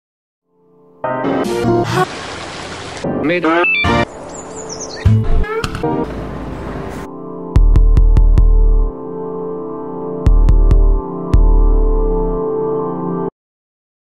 Kansa / Puettuja tunnelmia
Kansa on tutkielma suomalaisuudesta.
Kansa tuo nousevat nimet esiin.
Kansa on elämyksellinen muoti- ja kulttuurinäytös.

Tule ja koe neljättä kertaa järjestettävä Dressing Mimian vuositapahtuma Turun konserttitalolla lauantaina 10.11. Tarjolla on huikeita vaatemallistoja upein teatraalisin ja audiovisuaalisin puittein, tanssia, musiikkia, ruokaa, juomaa sekä mahtavaa seuraa.